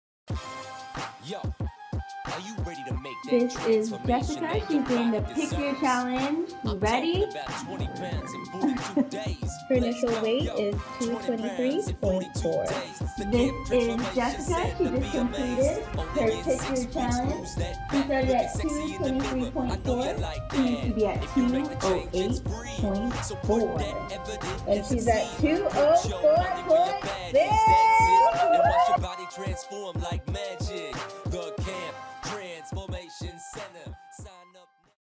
Chula Vista 6 Week Pick Your Challenge Results - Jessica Jefferson
We will now check the result of Jessica's 6 week PYC weight loss challenge!